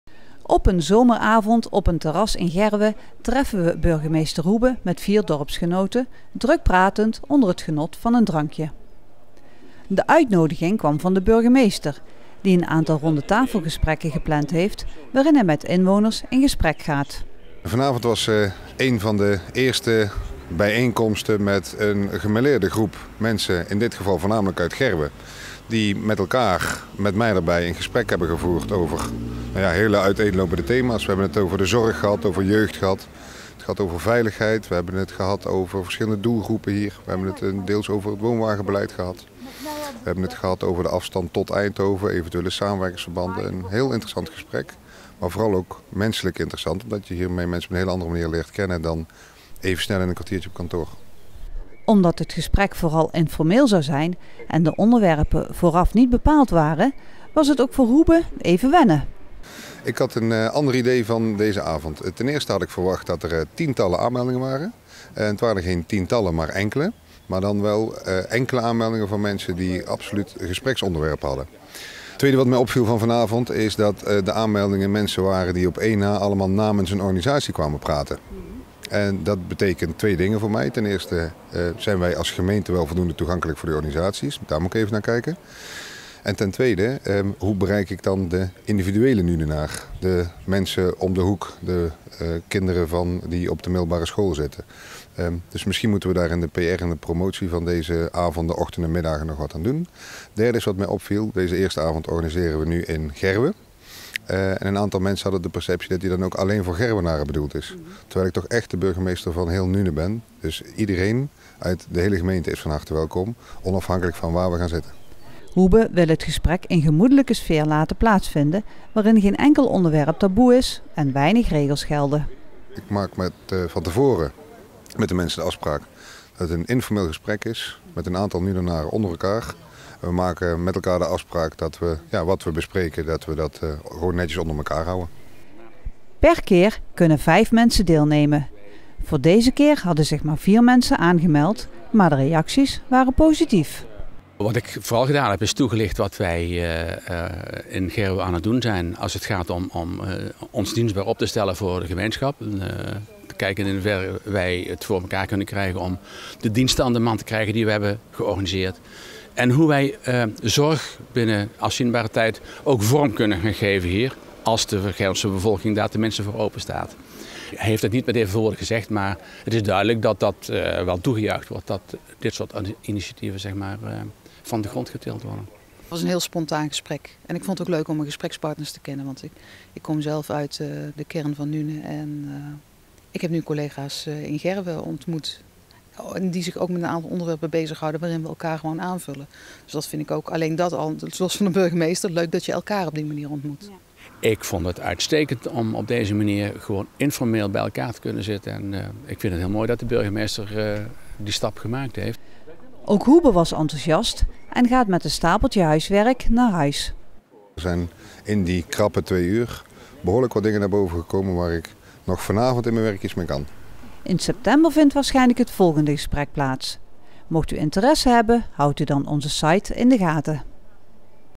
Burgemeester in Nuenen in gesprek met burgers
Onder de formule 'Rondje van d'n Burger' gaat burgemeester Maarten Houben in gesprek met inwoners van Nuenen. Zij kunnen bij hem terecht met ideeen, meningen en toekomstvisies. Het eerste gesprek vond op 20 juni plaats in restaurant De Drie Gebroeders in Gerwen.